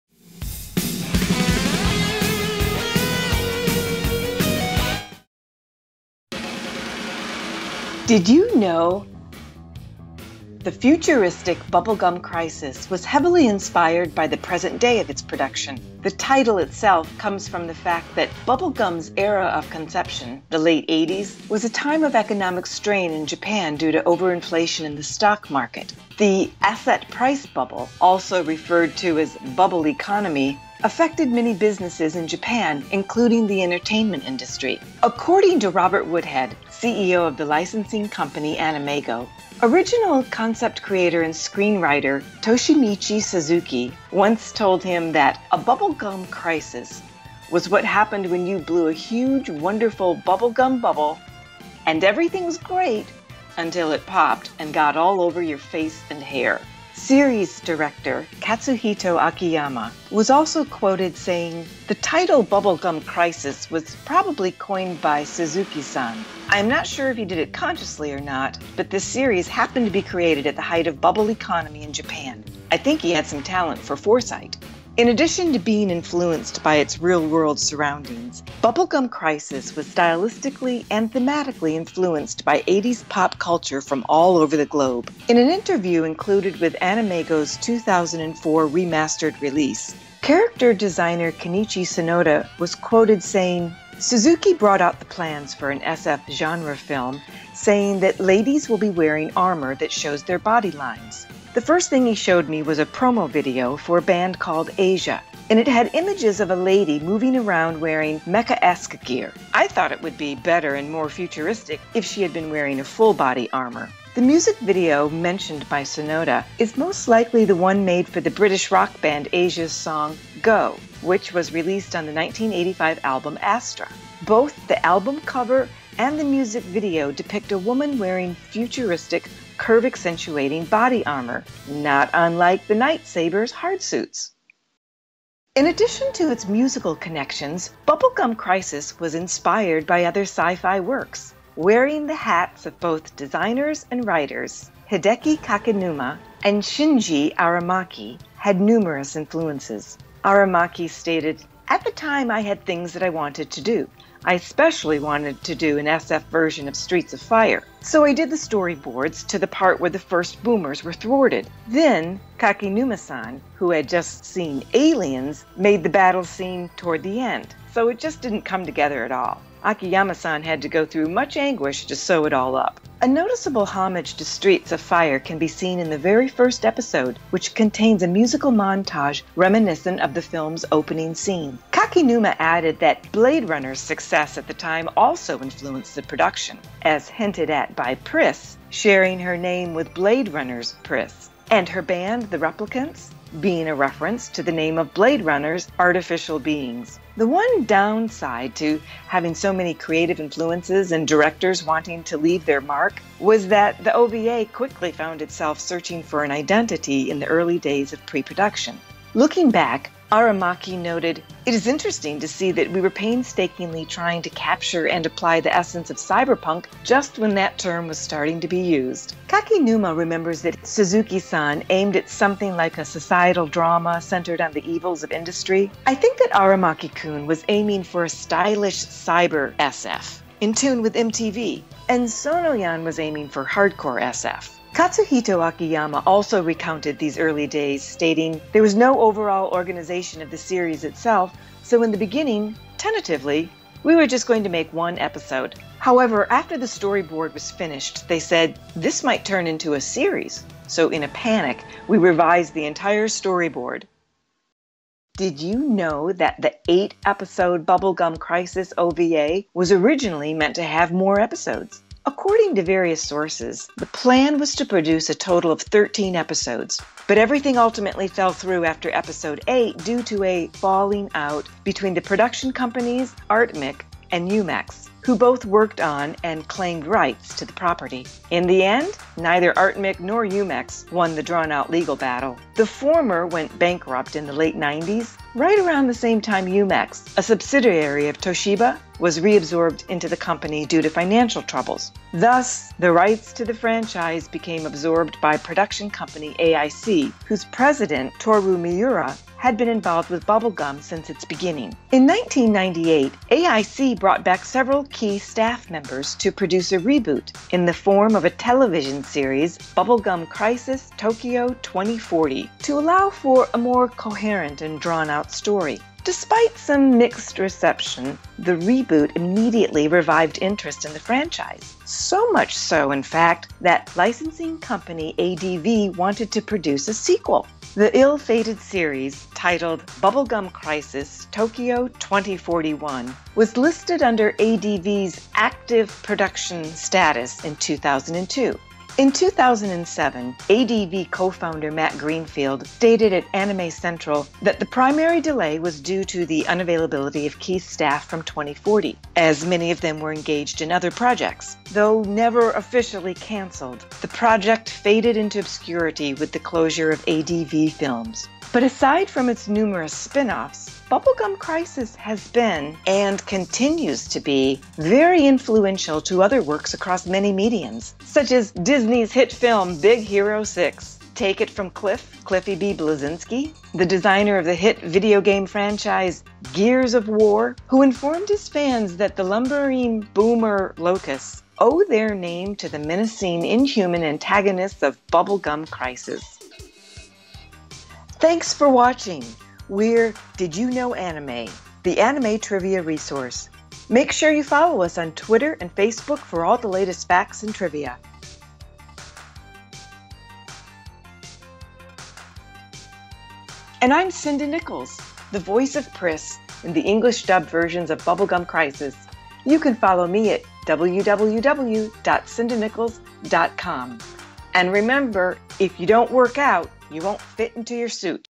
Bubblegum Crisis - Did You Know Anime? Feat. Sinda Nichols (Priss Asagiri)
Narrated by Sinda Lee Nichols, the legendary English voice of Priss, Did You Know Anime suits up for the chaos of Bubblegum's production, what influenced the crisis, and the long lost 2041!

Ben Barker
Phillip Johnson
xXxStoner420BongMasterxXx
Daniel A. Calix

T-shirts now available!